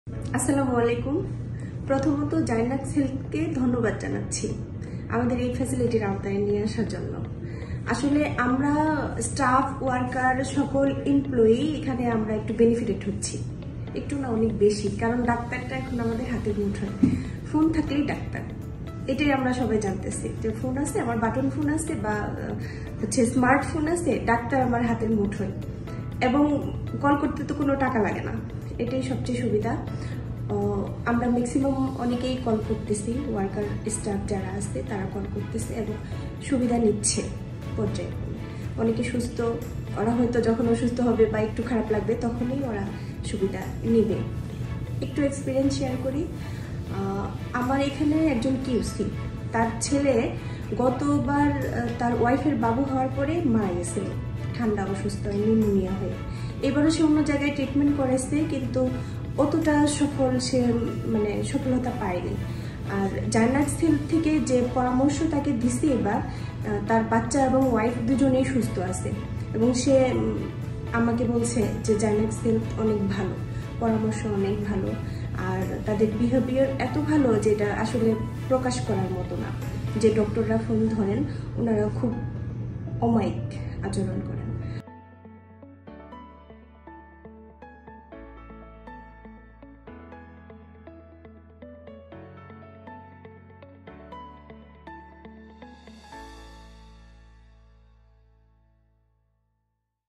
Client Feedback- South East Sweater Limited
বাংলাদেশের অন্যতম স্বাস্থ্যসেবা প্রদানকারী প্রতিষ্ঠান জায়ন্যাক্স হেলথ অন্যতম বৃহৎ তৈরী পোশাক রপ্তানী কারক প্রতিষ্ঠান সাউথ ইস্ট সোয়েটার লিমিটেড এর সাথে যুক্ত হয়েছে তাদের হেলথকেয়ার পার্টনার হিসেবে। লুতফন্নাহার শশী, সিনিয়র এক্সিকিউটিভ, ওয়েলফেয়ার, সাউথ ইস্ট সোয়েটার লিমিটেড শেয়ার করছেন জায়ন্যাক্স হেলথ এর ২৪/৭ অডিও-ভিডিও কলে বিশেষজ্ঞ ডাক্তার সার্ভিস এবং ক্যাশব্যাক নিয়ে তার সন্তুষ্টির কথা।

ধন্যবাদ লুতফন্নাহার শশীকে আমাদের সার্ভিস সম্পর্কে তার সন্তুষ্টির কথা জানানোর জন্য।

আপনার কোম্পানীর সকল কর্মকর্তা ও কর্মচারীদের স্বাস্থ্য সুরক্ষায় আপনিও যুক্ত হতে পারেন জায়ন্যাক্স হেলথ এর সাথে।

বিস্তারিত জানতে কল করুন আমাদের টোল-ফ্রি নাম্বারেঃ ০৮০০ ৮৮৮ ০০০০